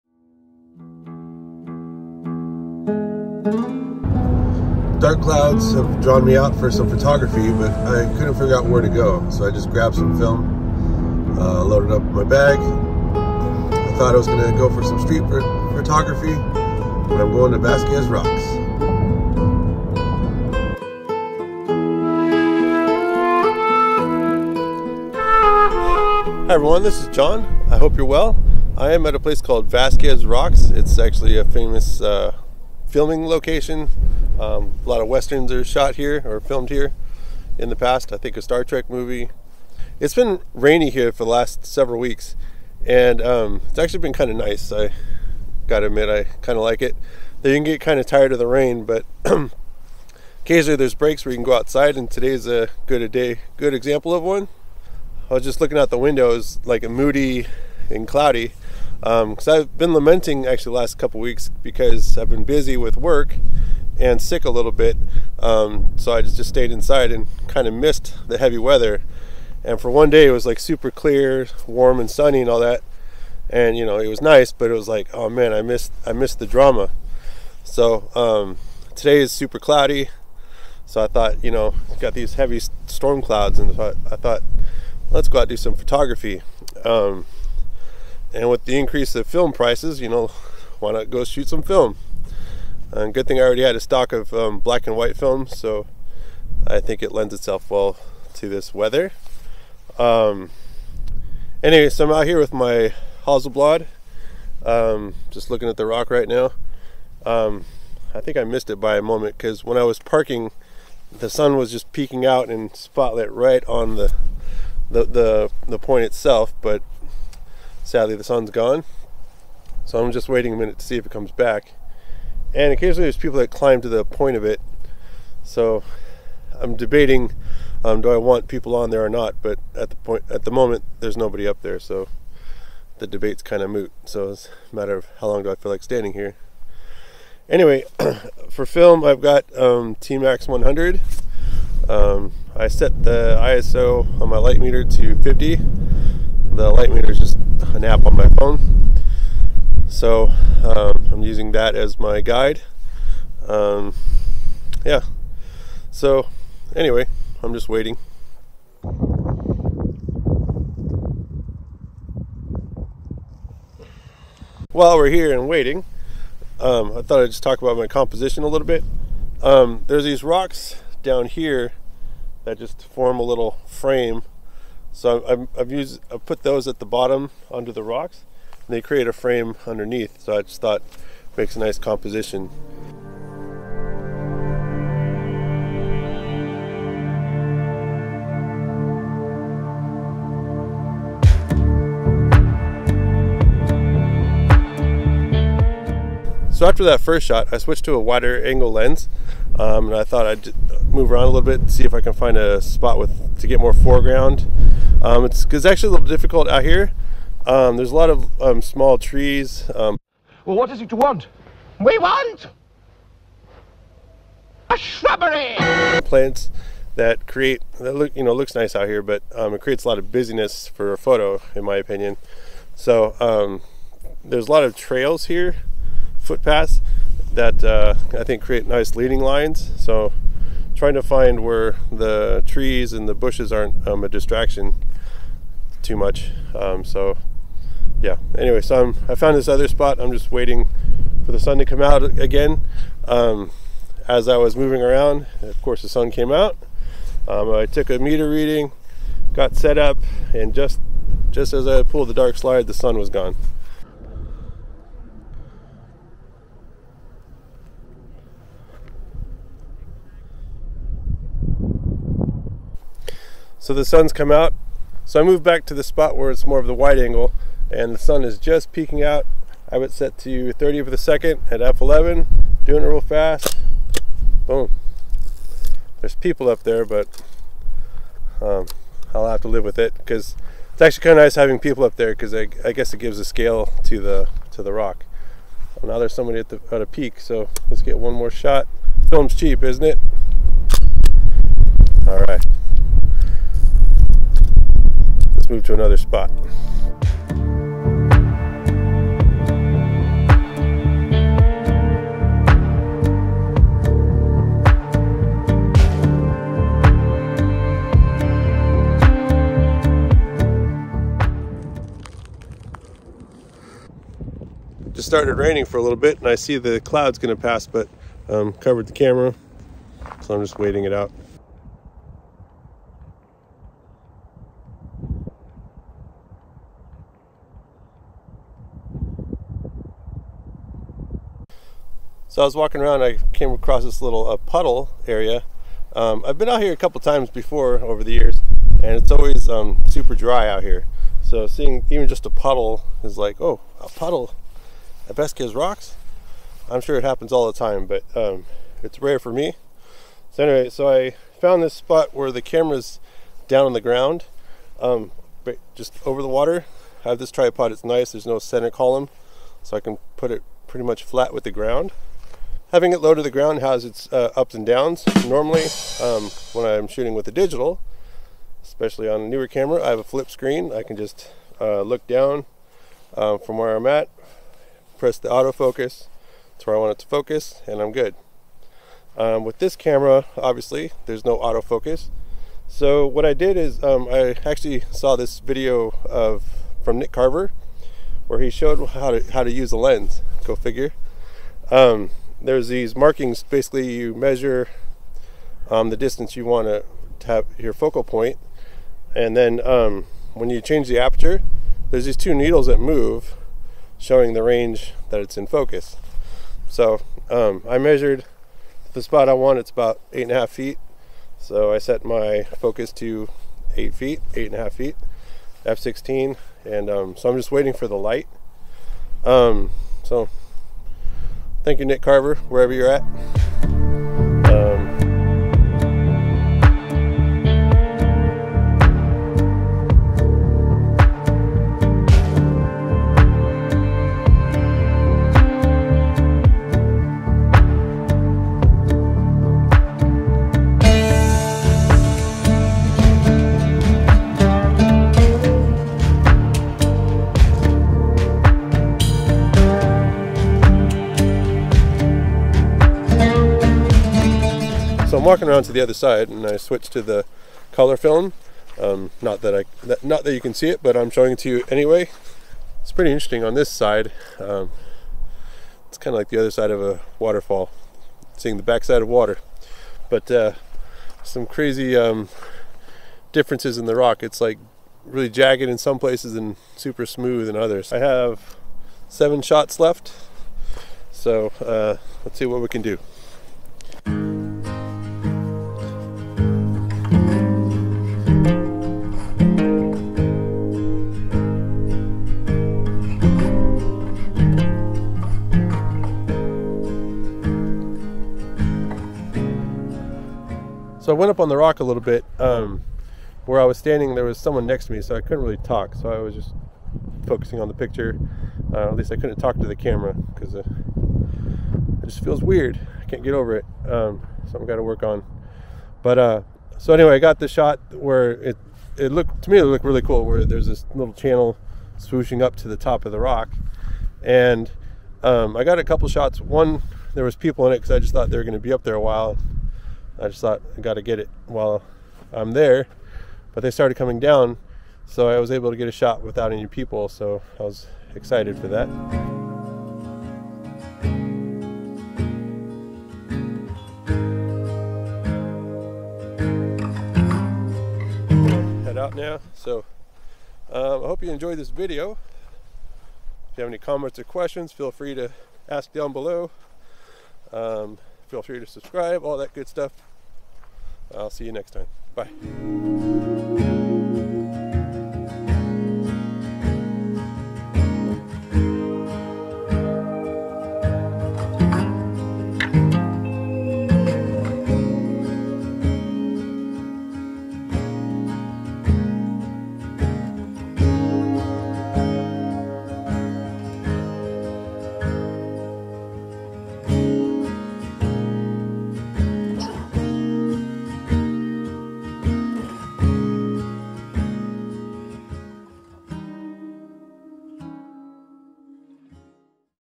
Vasquez Rocks | Hasselblad 500CM | Kodak TMAX 100
For nearly 2 months we in California we had non-stop rain. For half of it I was too busy to enjoy it because of work...a good problem. Finally with some free time, the moody clouds were calling me out for some black and white photography. In this video I head to Vasquez Rocks with my Hasselblad. Enjoy!

𝑮𝒆𝒂𝒓 𝑼𝒔𝒆𝒅
Hasselblad 500CM with 60mm and 150mm lenses
Kodak TMAX 100
Kodak Portra 400 (the last photo)
DJI Pocket 2 (trying it for the first time)